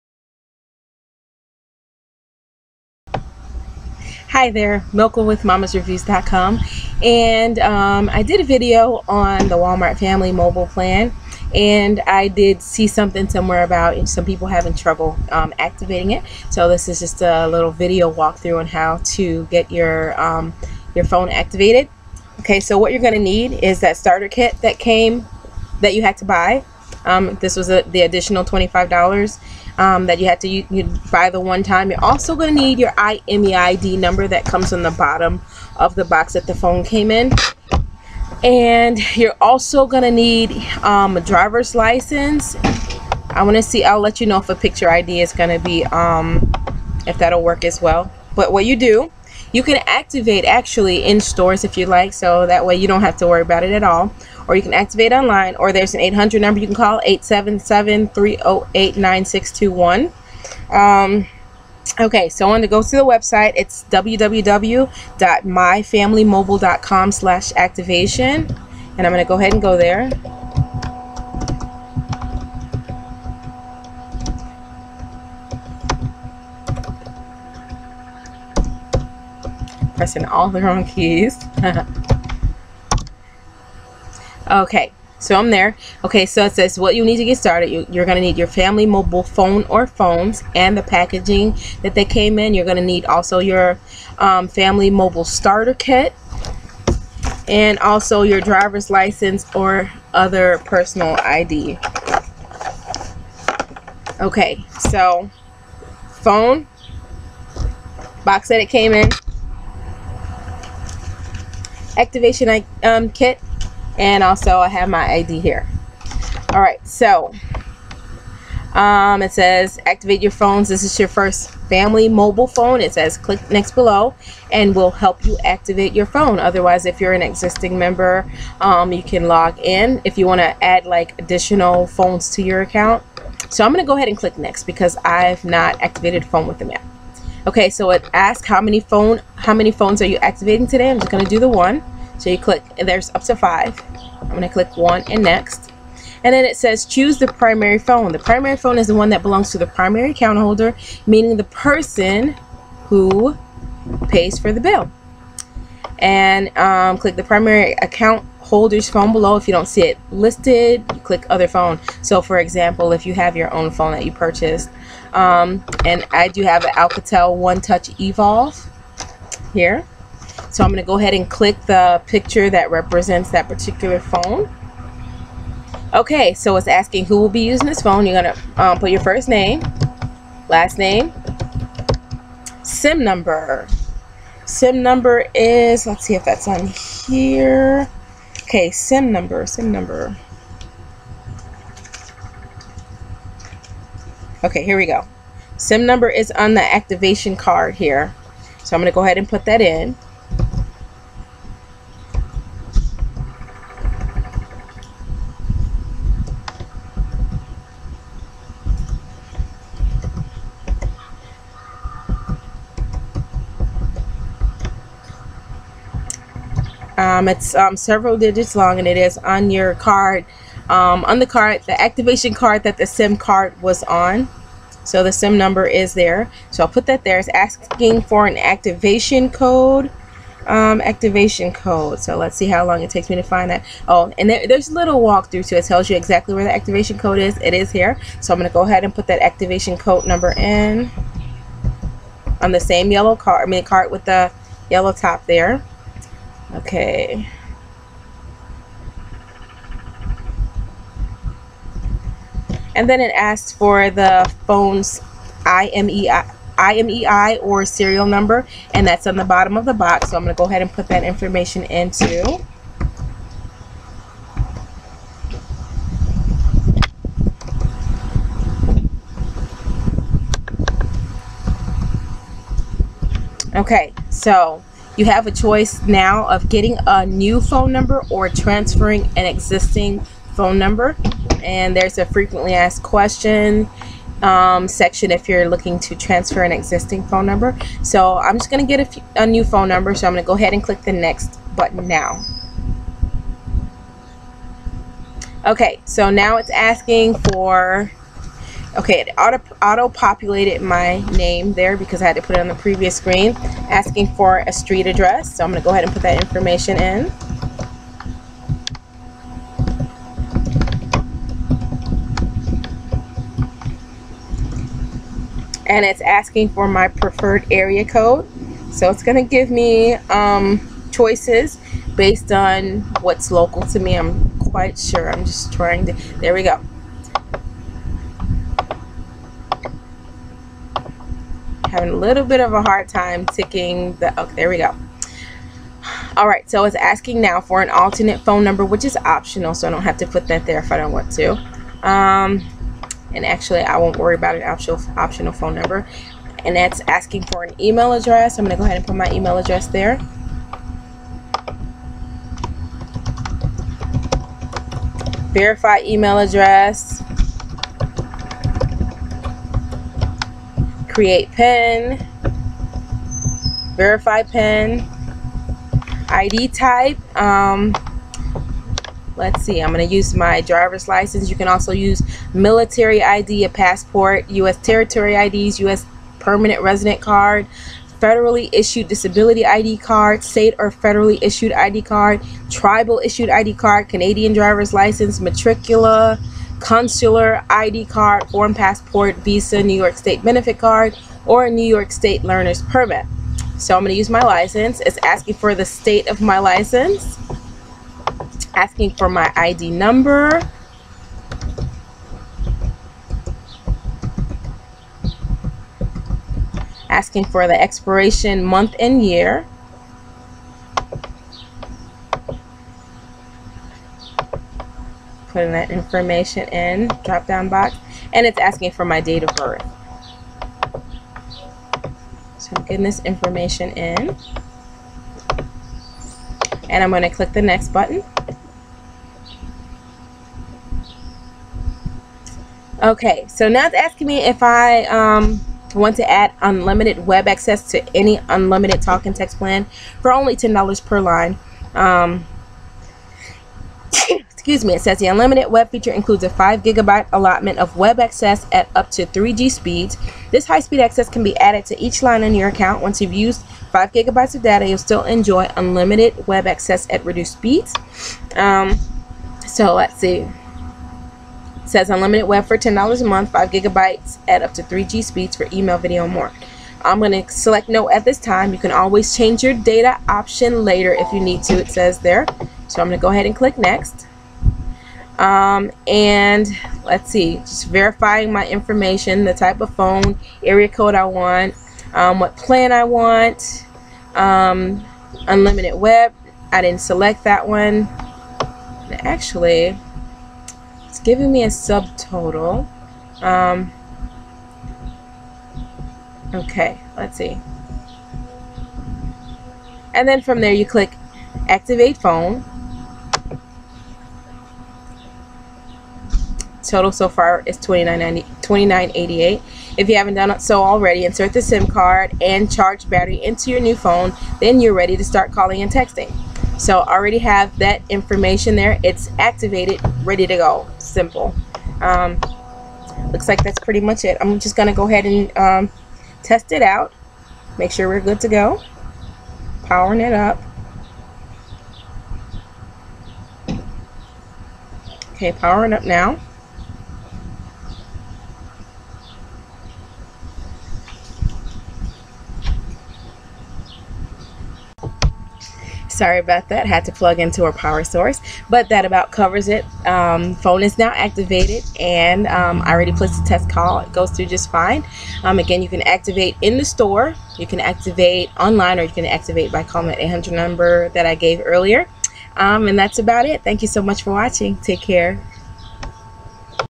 How to activate your Walmart Family Mobile phone.
Walk through for activating your newly purchased Walmart Family Mobile phone.